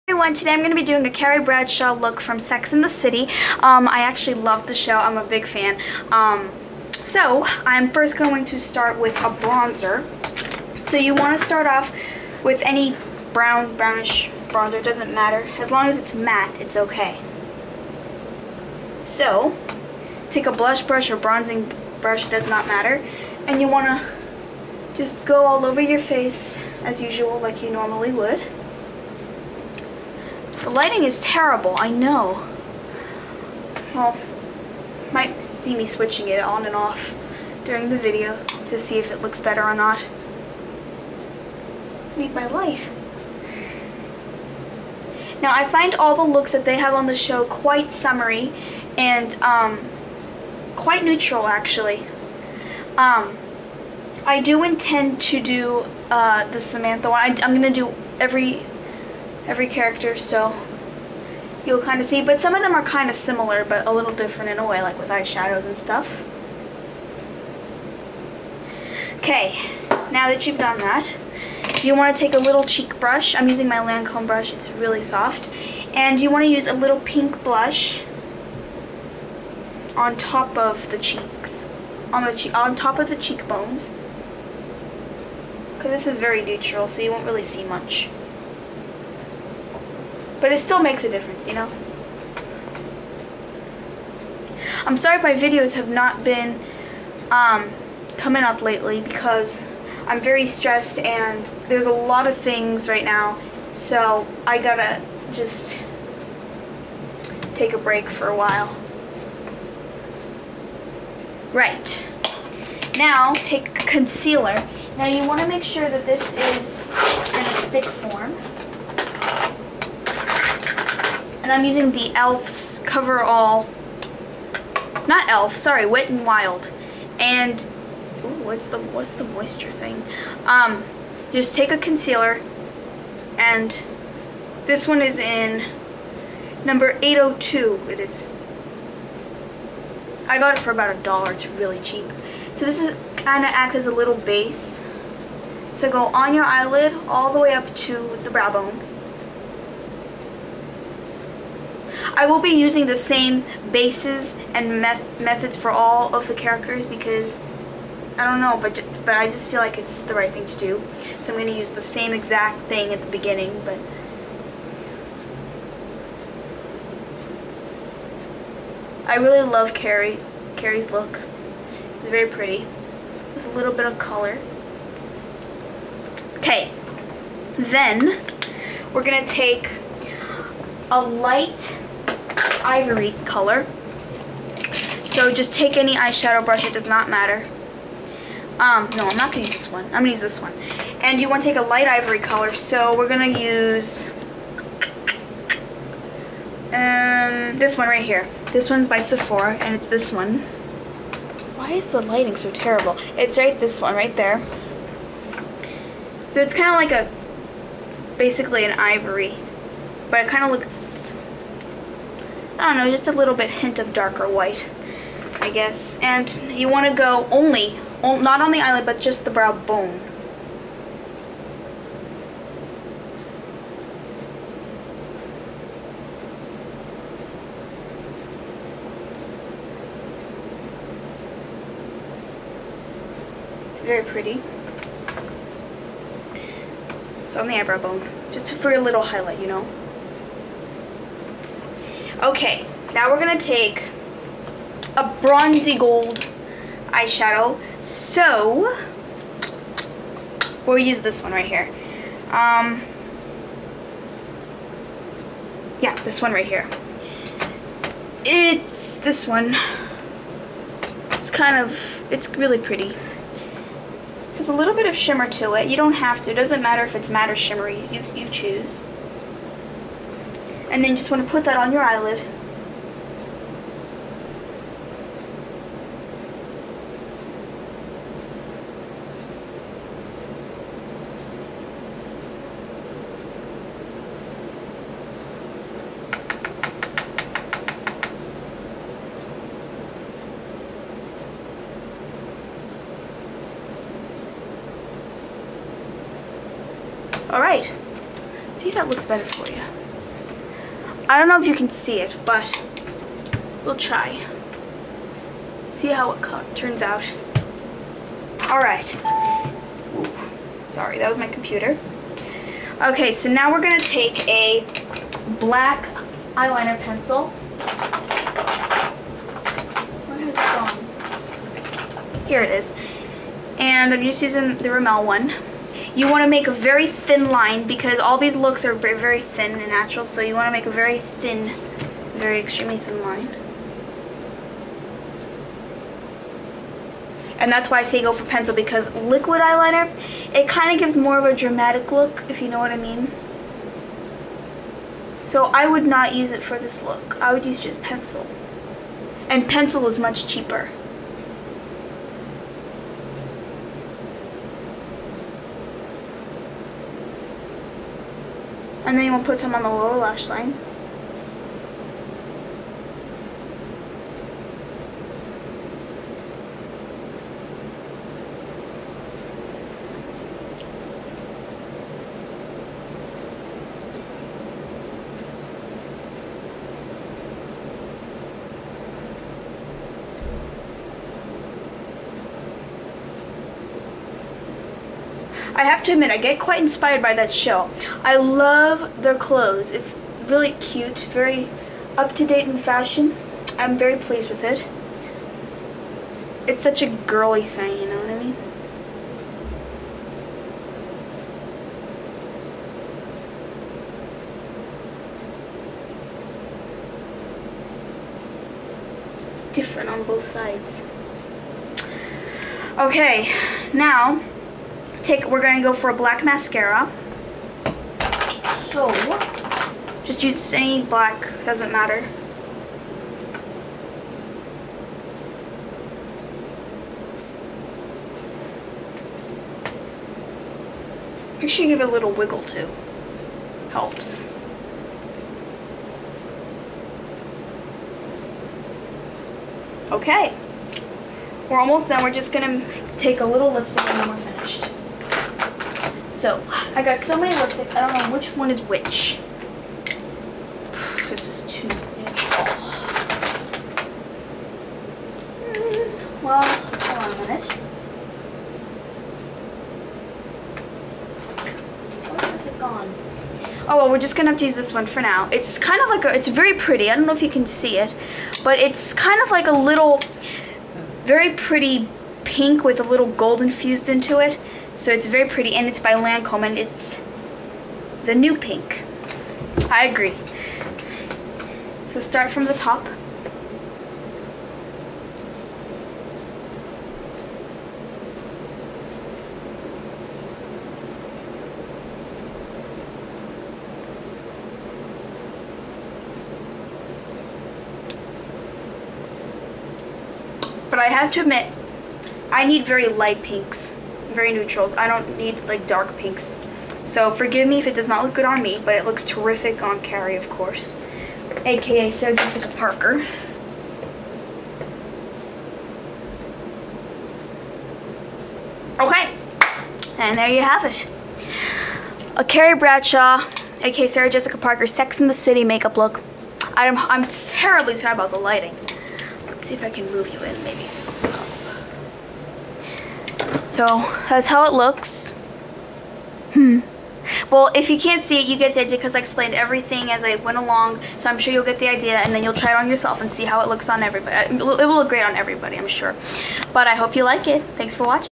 Carrie Bradshaw "Sex And The City" Makeup Look
Products Used:
Sephora Bronzer
Sephora Pink Blush
Wet 'n' Wild- Cover All Concealer
Sephora Light Ivory eyeshadow
Bronze-Gold Sephora eyeshadow
Rimmel- Black Eyeliner Pencil-Black Magic
blinc- Black Mascara
Lancome Lipstick- The New Pink